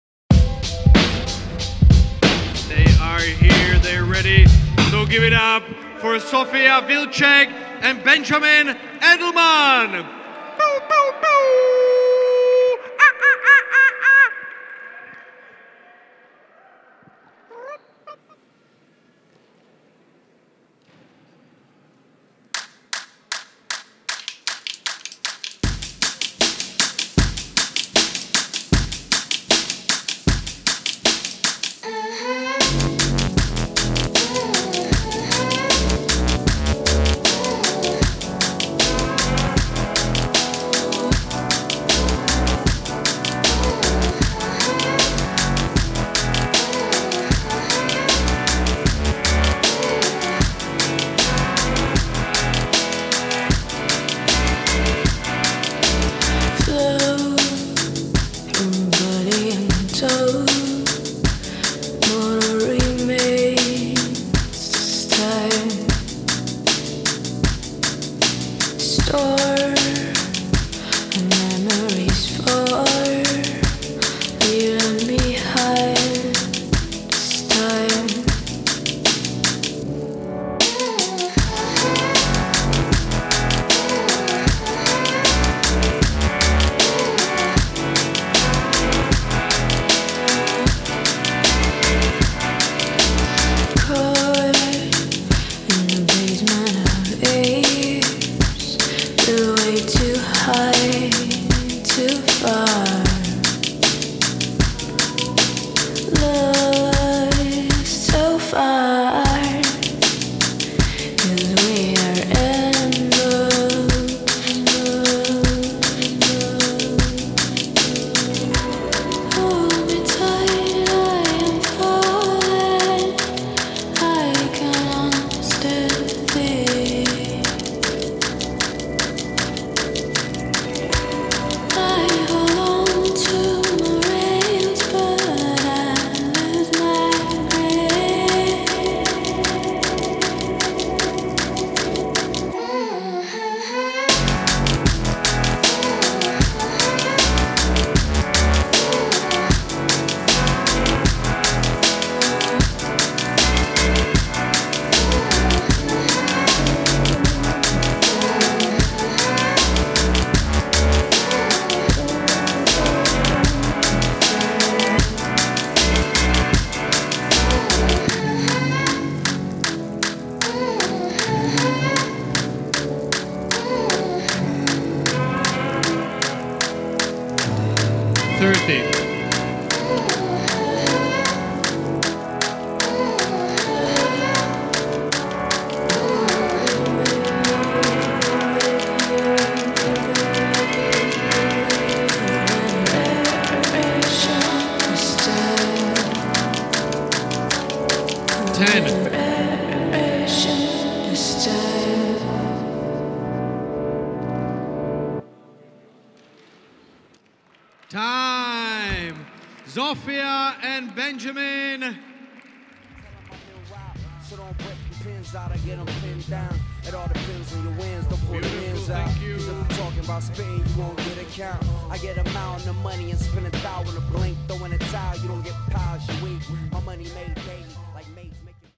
Mixed Pairs Finals - Zofia Wilczek & Benjamin Edelmann | Frisbeer Cup 2023
5th place Mixed Pairs routine at Frisbeer Cup 2023